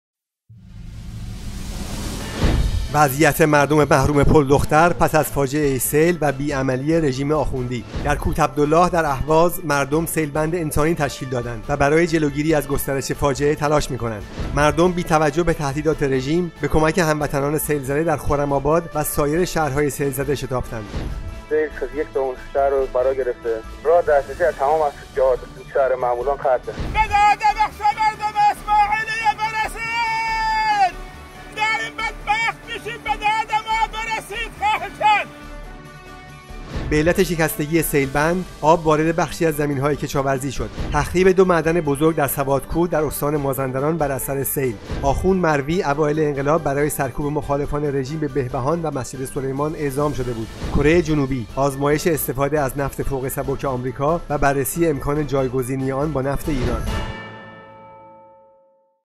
مهمترین اخبار ایران و جهان در ۶۰ثانیه اخبار شامگاهی ۱۵ فروردین ۹۸
وضعیت به‌شدت بحرانی شهر پلدختر

وضعیت مردم محروم پلدختر پس از فاجعه سیل و بی‌عملی رژیم آخوندی.

سیل‌بند انسانی

در کوت عبدالله در اهواز مردم سیل‌بند انسانی تشکیل دادند / و برای جلوگیری از گسترش فاجعه تلاش می‌کنند.

کمک‌رسانی بی‌دریع مردم به سیل‌زدگان لرستان

مردم، بی‌توجه به تهدیدات رژیم، به کمک هموطنان سیل‌زده در خرم‌آباد و سایر شهرهای سیل‌زده شتافتند

اعتراف شهردار رژیم در معمولان به تخریب گسترده

سیل یک‌سوم شهر را فراگرفته، راه دسترسی از تمام جهات به شهر معمولان قطع است

فریاد کمک خواهی یک هموطن اهوازی

به داد مردم بخش اسماعیلیه برسید. داریم بدبخت میشیم... خواهشا...